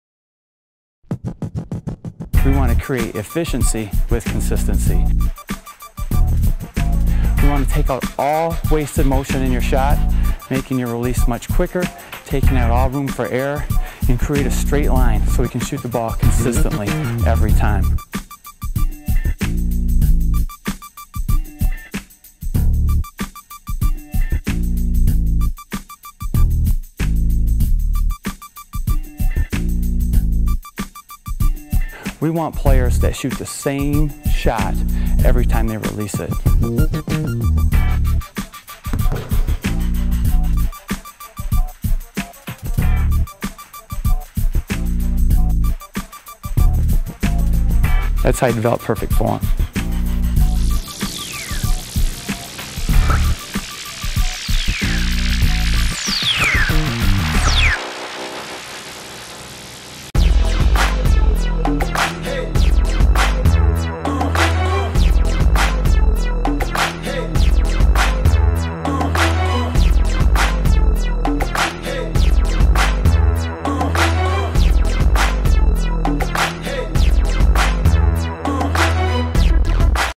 Perfect Jump Shot Form & Technique with Jason Otter
Build your shot from the ground up with the Perfect Jump Shot. Use the techniques and drills on the DVD to build your perfect jump shot. If you are a parent looking to help your child out, this DVD will be your blueprint to your child's perfect jump shot. If you are an advanced player looking to perfect your shot. This DVD features exciting presentation and is fun to watch. No fluff filled segments on this DVD as Jason will give you easy to follow instruction along with the best drills to do to perfect your jump shot.

For beginning and advanced players
Build your shot from the ground up
Multiple camera angles, on screen graphics, & exciting presentation
Easy to follow instruction
Great for individuals and coaches